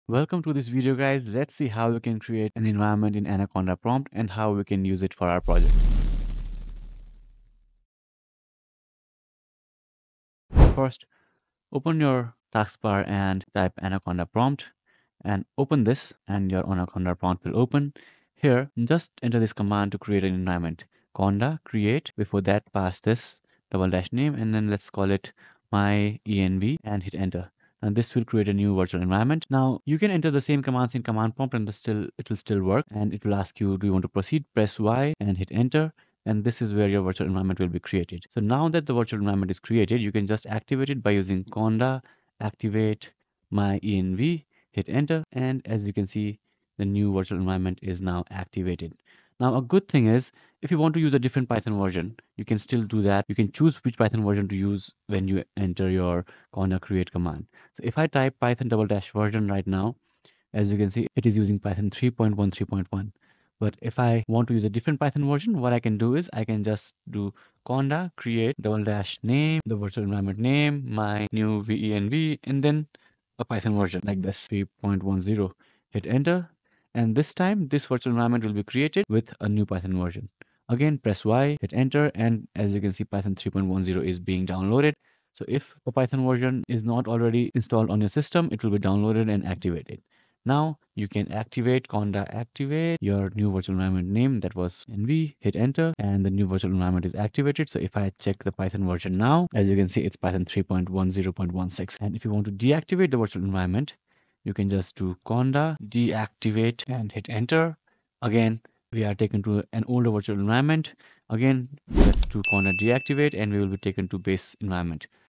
How to Create Python Environments in Anaconda Prompt (2025 Tutorial)
Setting up Python environments in Anaconda Prompt has never been easier! In this step-by-step tutorial, you'll learn how to create and manage virtual environments using Conda. Perfect for beginners and experienced developers alike, this guide will help you set up a clean and organized workspace for your Python projects. 🚀

👉 Why should you watch this?
Whether you're working on machine learning, data science, or web development, having separate environments for your projects ensures everything runs smoothly.
Mastering Conda will save you time and headaches while working with Python libraries.